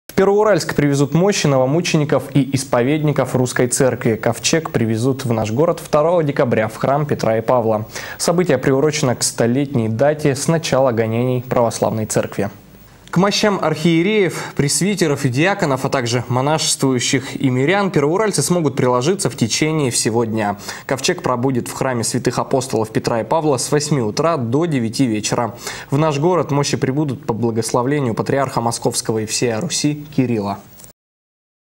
В Первоуральск привезут святые мощи
Ковчег прибудет в наш город 2-го декабря, в храм Петра и Павла. Событие приурочено к 100-летней дате с начала гонений православной церкви.
К мощам архиереев, пресвитеров и диаконов, а так же монашествующих и мирян первоуральцы смогут приложиться в течение всего дня. Ковчег пробудет в Храме святых апостолов Петра и Павла с 8 утра до 9 вечера. В наш город мощи прибудут по благословлению патриарха Московского и всея Руси Кирилла.